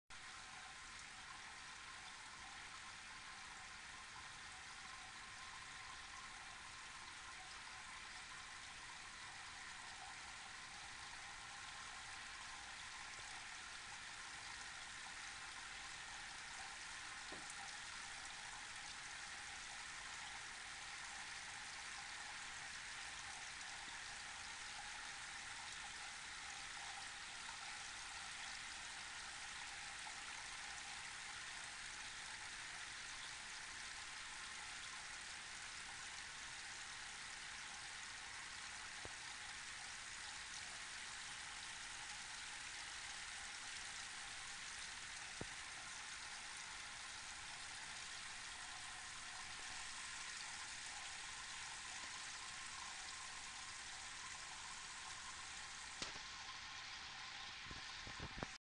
Diamond Rain Fountain
"Diamond Rain Fountain," by Charles McBride White, sounds like a gentle rain and produces a slight mist.  Maintenance is very simple.  It provides an elegant focal point in your home, office, or wine cellar.